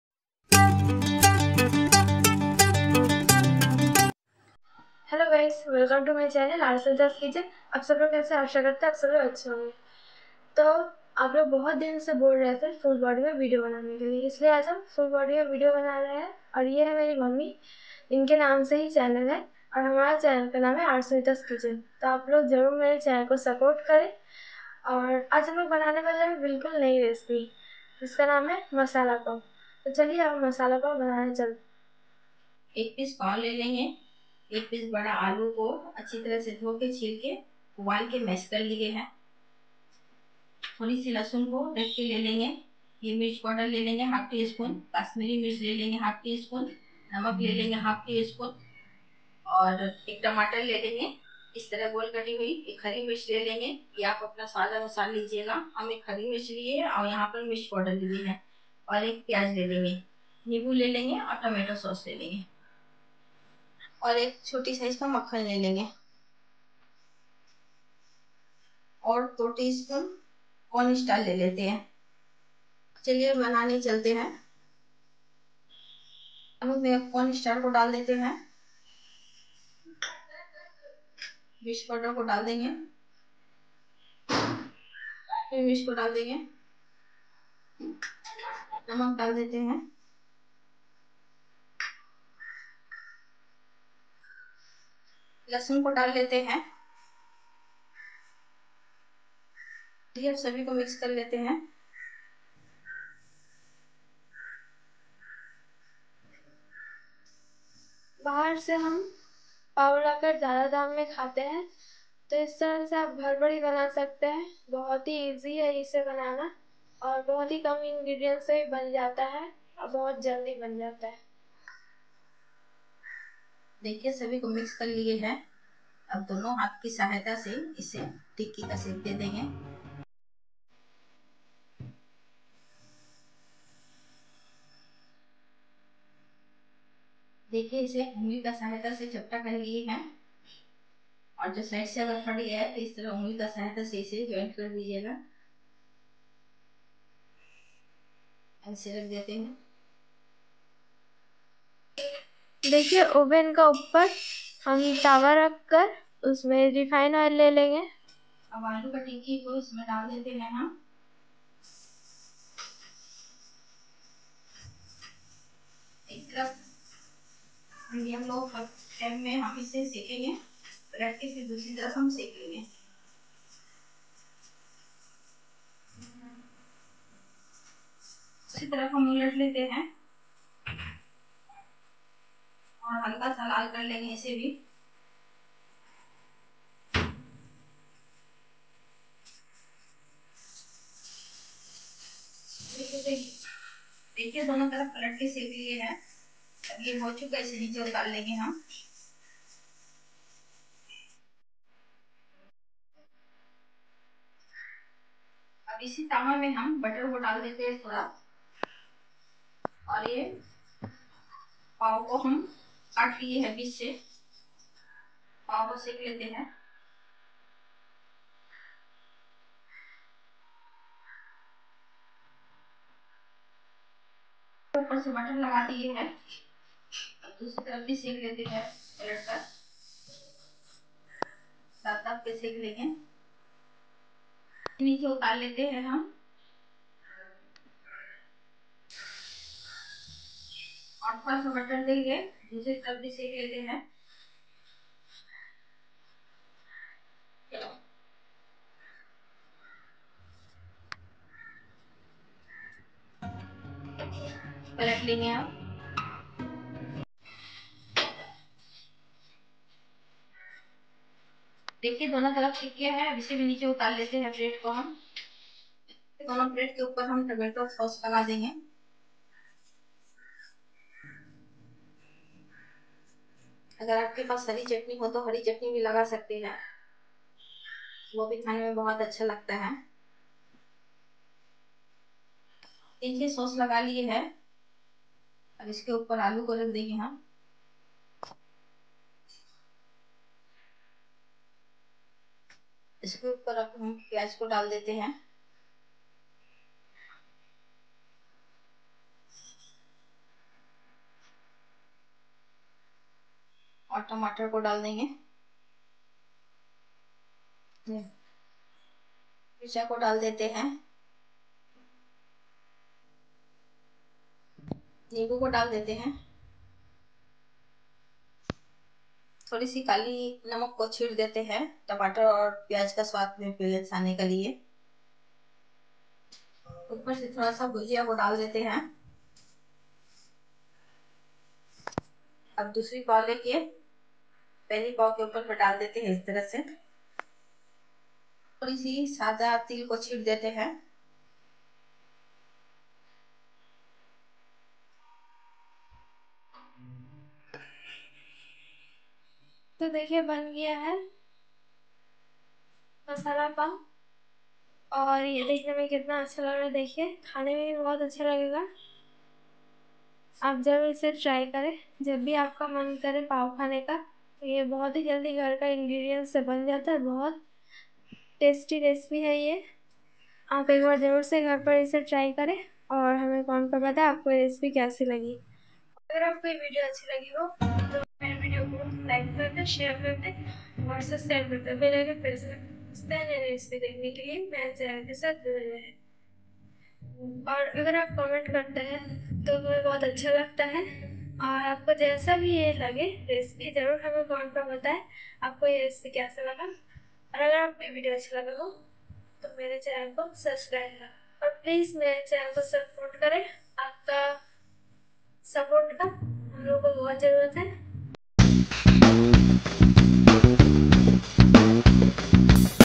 Ghar ke hi ingredients se banaye masala pav
1 piece pav, 1 piece bara aalu, lasun, ½ teaspoon mirch powder, ½ teaspoon kasmiri mirch, ½ teaspoon namak, 1 tamatar, 2 hari mirch, 1 pyaz, nimbu, tomato ketchup, ¼ teaspoon sada til, 2 teaspoon bhujiya, 1 pinch kala namak.